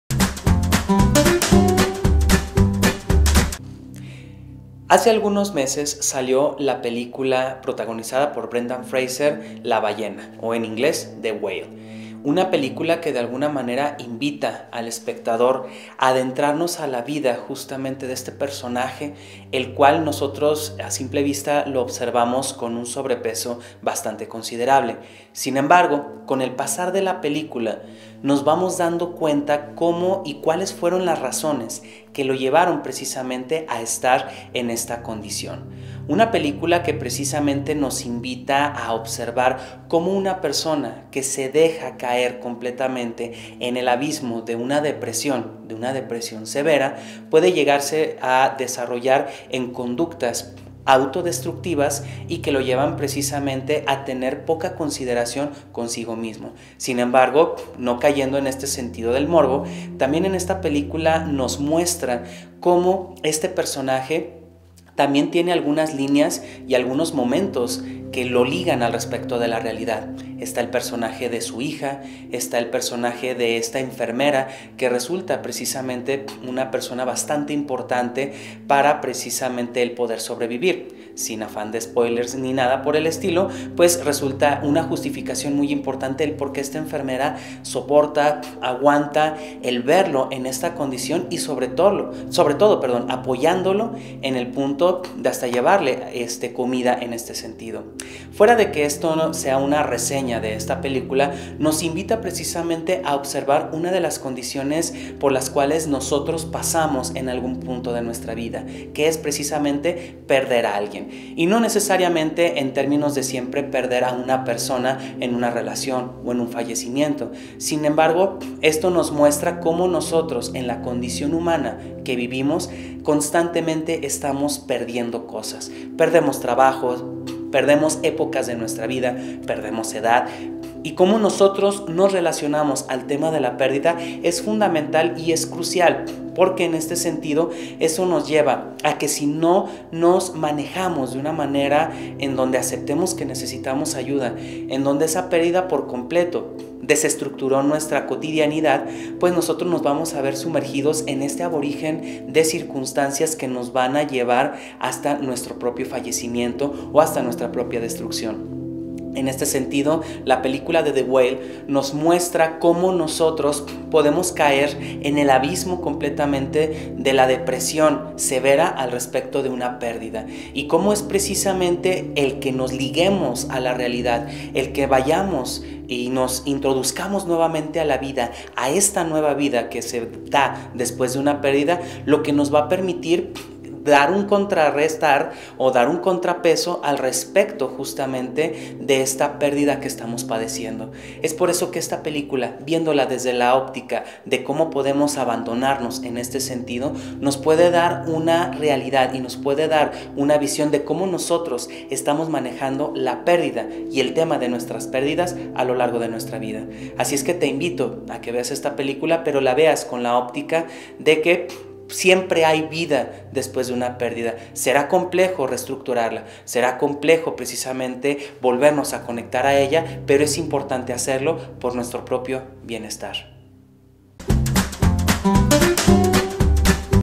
🟢La Ballena 🐋 Análisis psicológico por el Dr Eumir Cázares
Eumir Cázares hace un recuento de los temas tratados en esta película ganadora del Óscar -depresión, pérdidas, recuperación, apoyo- desde el punto de vista de la psicología.